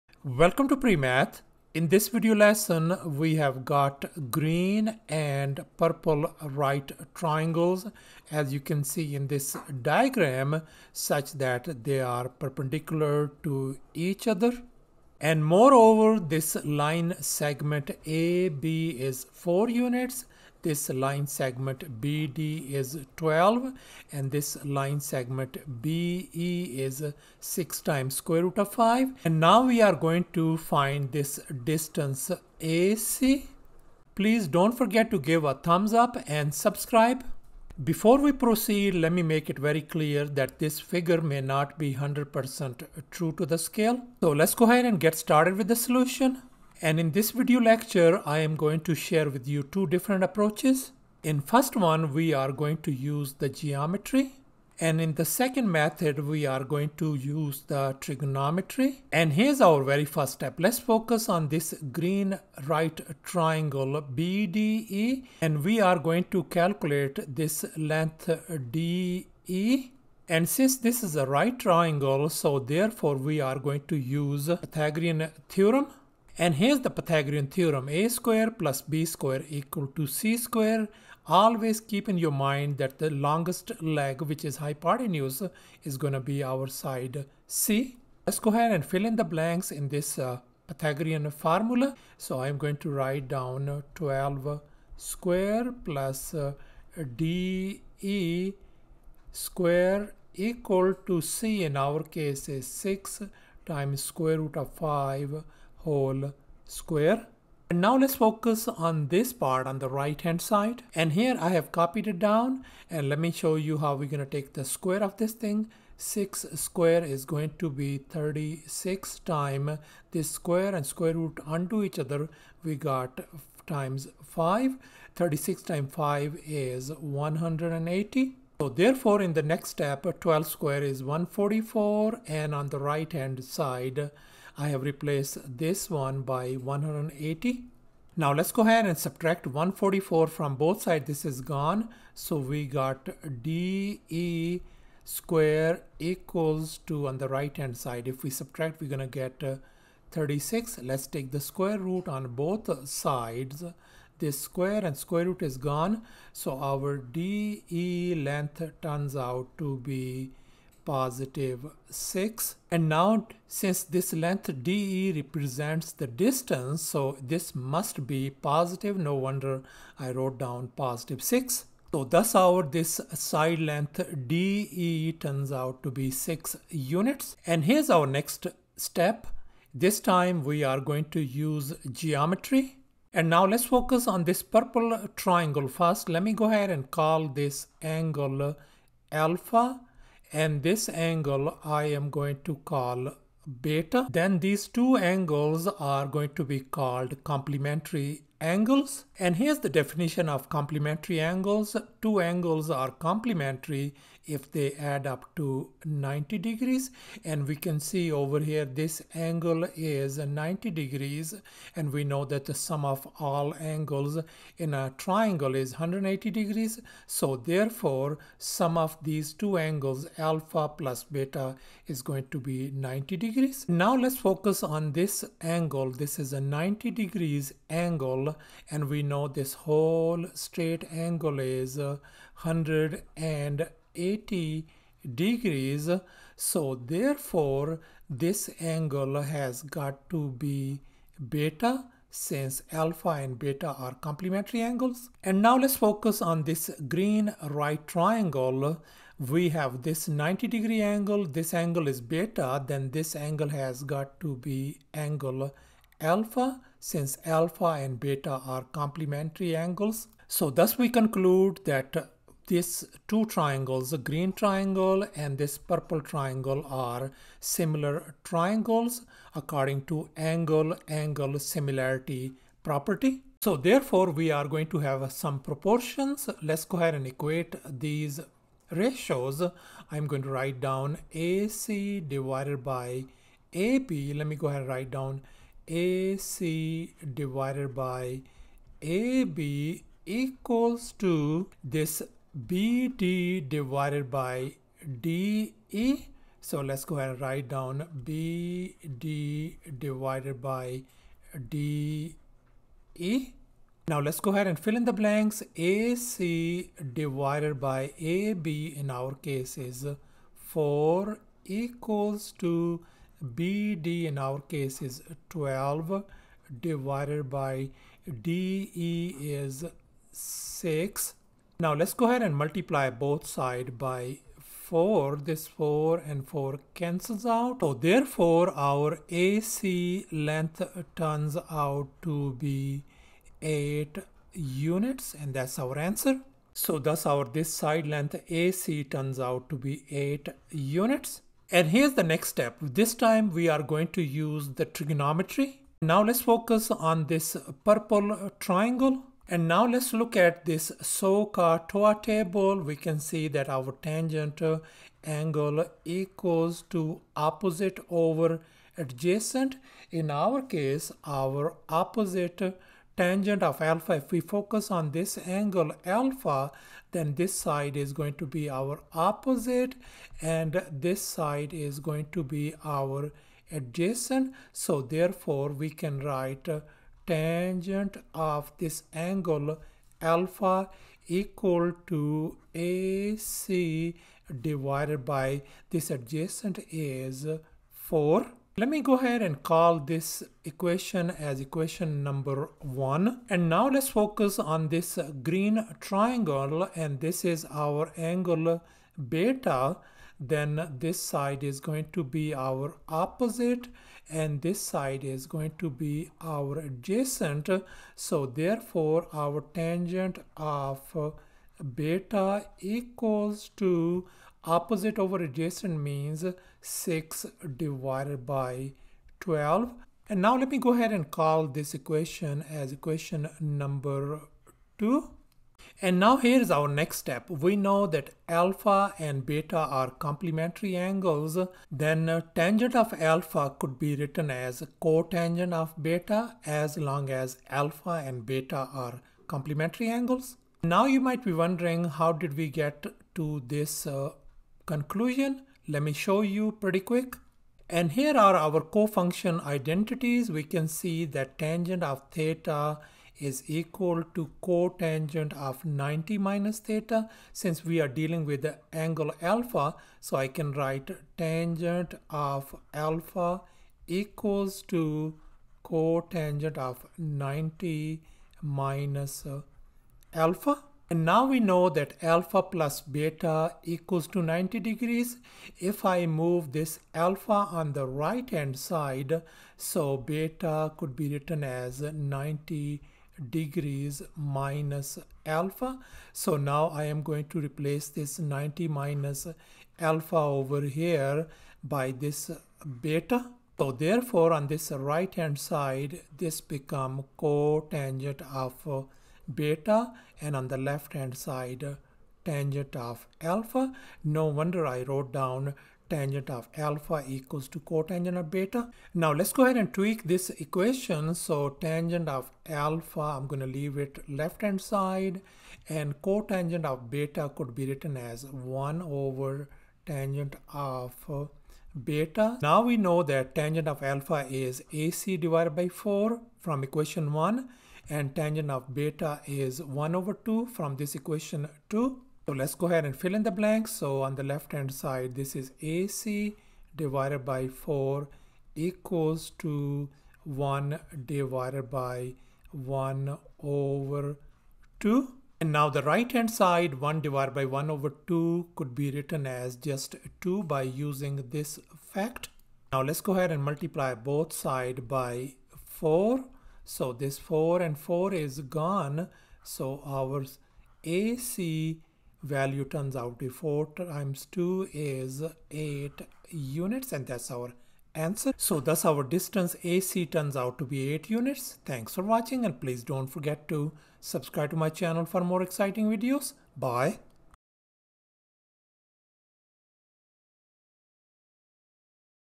Can you find the Distance AC? | Two Easy Methods | Geometry Challenge
Learn how to find the distance AC when given 2 right triangles. Solve by using the Pythagorean Theorem, complementary angles, and the angle-angle similarity property. Step-by-step tutorial by PreMath.com